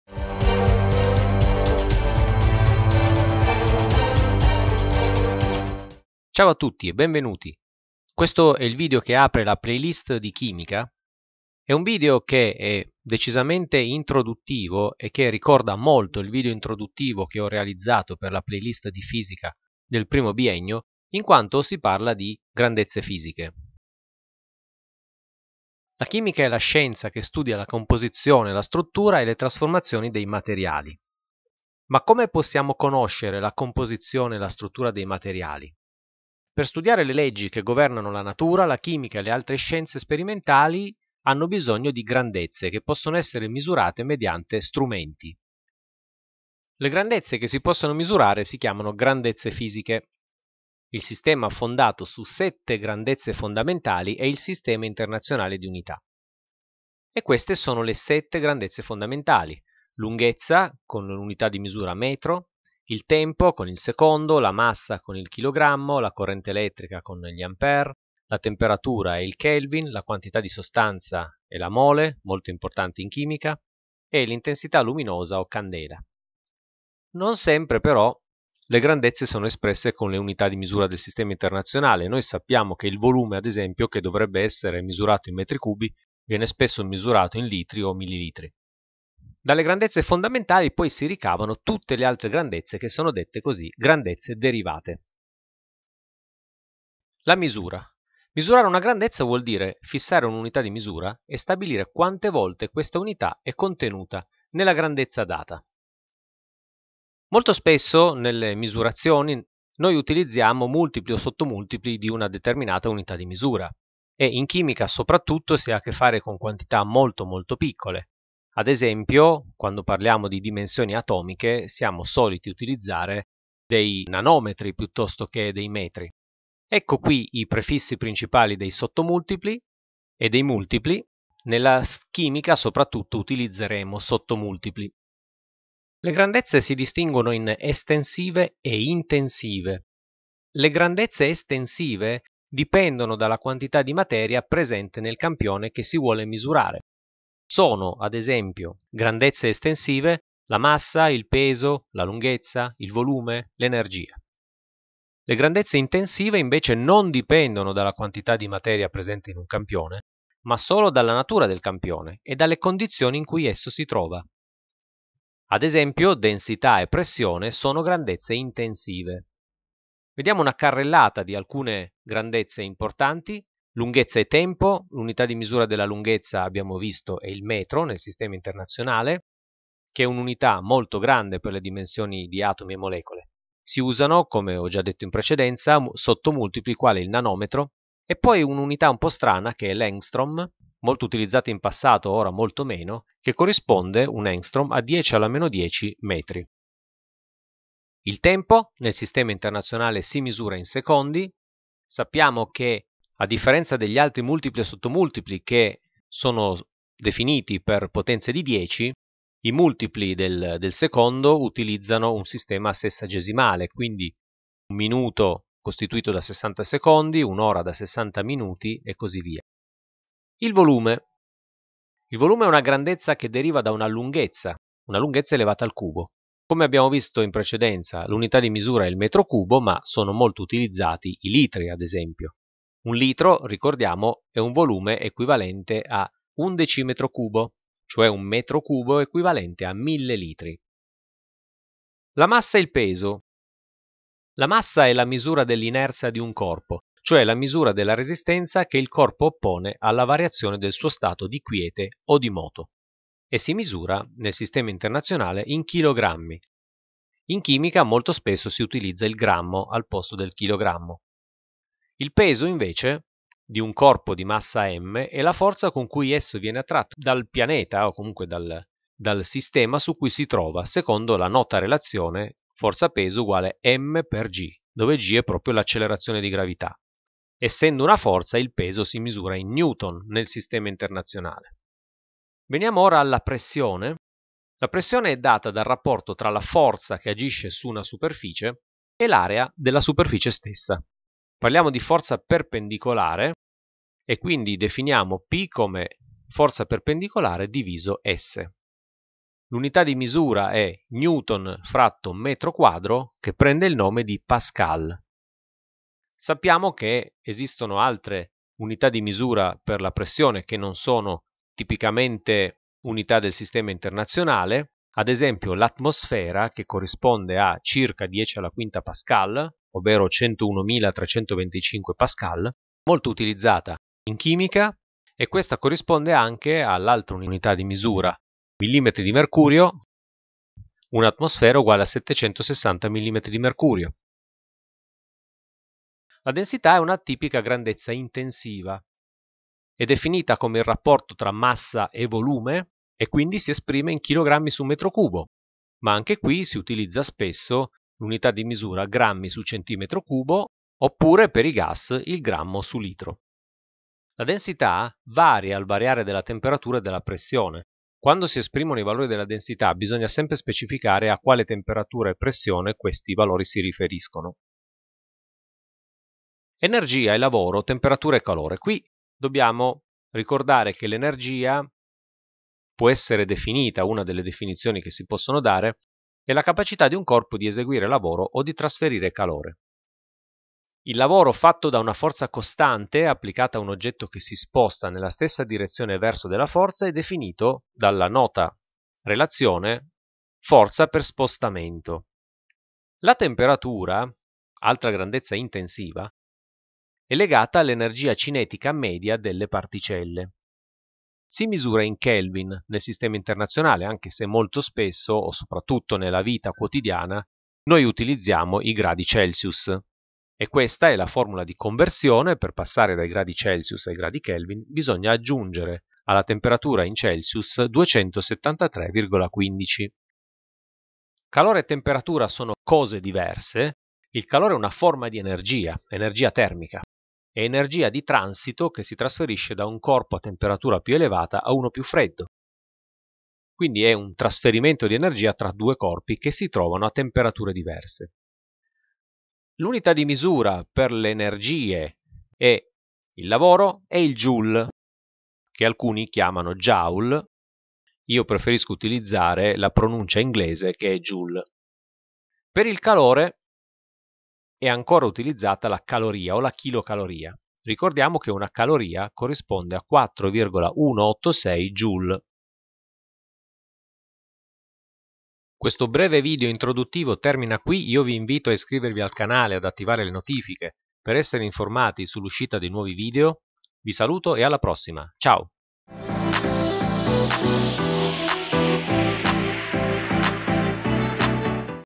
Introduzione alla chimica - Misure e grandezze - Chimica p.1
I MIEI STRUMENTI:
Chimica.blu. Dalla materia alle proprietà periodiche.
Chimica: concetti e modelli. Volume unico.
Chimica: molecole in movimento. Vol. unico.
Tavoletta grafica Wacom One
Questo è un video di introduzione alla chimica.
Si parla di sistema internazionale di unità di misura, di multipli e sottomultipli delle unità di misura, di grandezze estensive ed intensive.
0:00 Intro
0:24 La chimica
0:46 Sistema Internazionale di Unità
1:36 La misura
2:15 Grandezze estensive
2:34 Grandezze intensive
2:49 Lunghezza e tempo
3:40 Volume
4:01 Massa e peso
4:42 Pressione
5:35 Densità
6:05 Energia, Temperatura, Calore